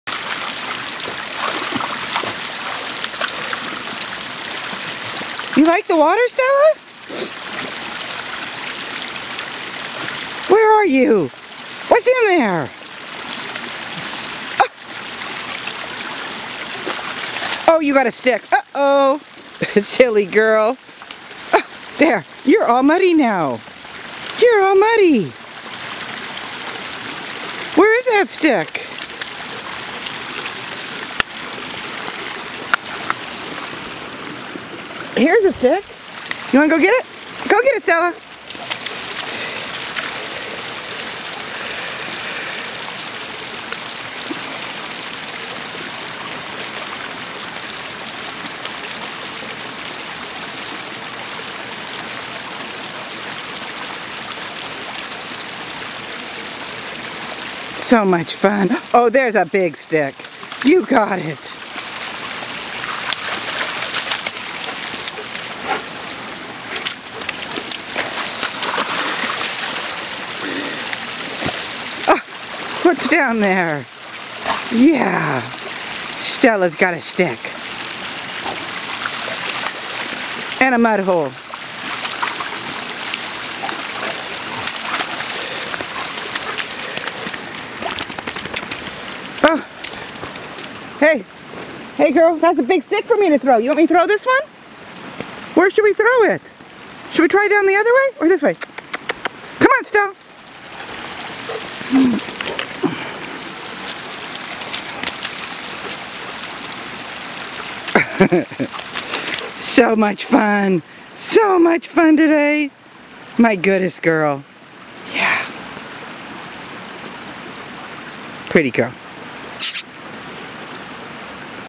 Adventure Oregon - Puppy playtime along backcountry Upper Smith Creek trail at Silver Falls
Adventure Oregon - Stella a Bernese Mountain dog had so much fun along the Backcountry Upper Smith Creek trail at Silver Falls State Park.  Enjoyed the trail yet never seeing anyone else on a Saturday afternoon.  Dog, horse, bike and kid friendly safe trails just 30 minutes from Salem Oregon.